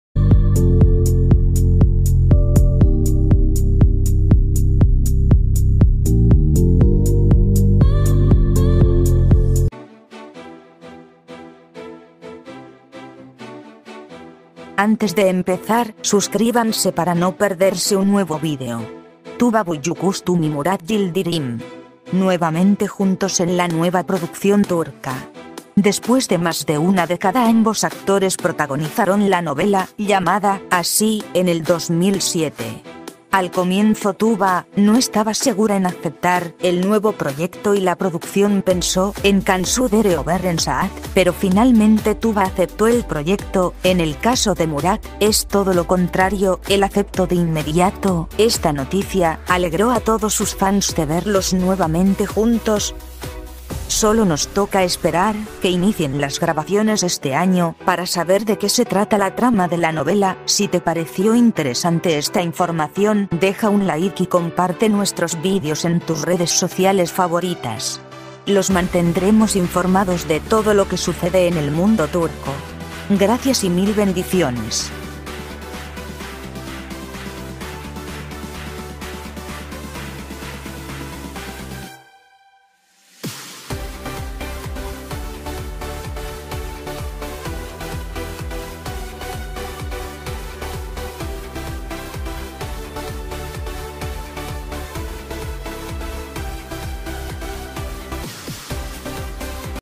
NUEVA SERIE TURCA (Tuba Büyüküstün y Murat Yıldırım )
Los actores Tuba Büyüküstün y Murat Yıldırım , en una nueva serie turca  , se iniciaran las grabaciones ya este 2018 , despues de mas de una decada nuevamente juntos